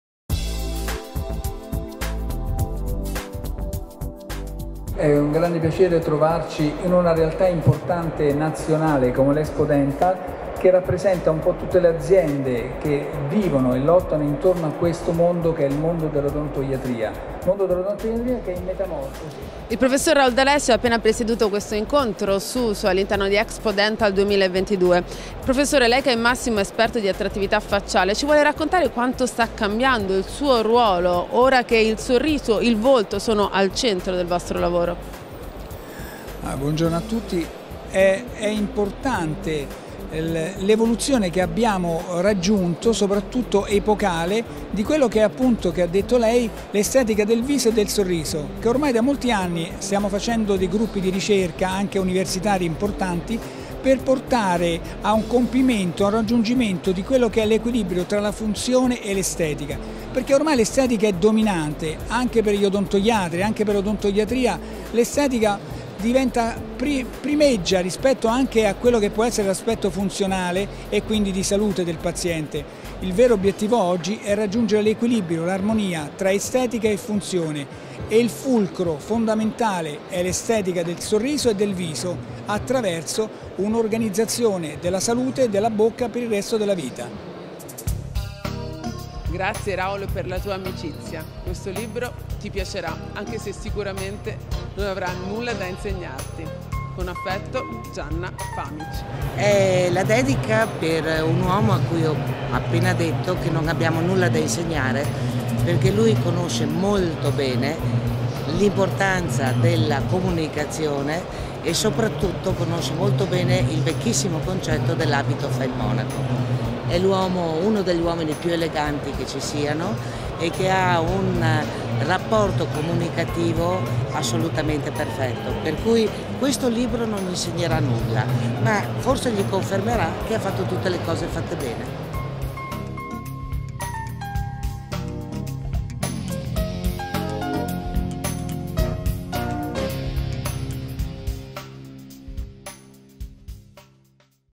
Prof. Raoul D'Alessio - Expo Dental Meeting 2022
Intervista su estetica del viso e del sorriso al Prof. Raoul D'Alessio – Medico Chirurgo specializzato in Ortognatodonzia, coordinatore dei Presidenti SUSO.  Intervista realizzata realizzata a Expo Dental Meeting 2022 (Rimini 19, 20 e 21 maggio). Nel video anche un intervento di Gianna Pamich, presidente Unione Nazionale Industrie Dentarie Italiane, autrice del libro "Effetto Bianco".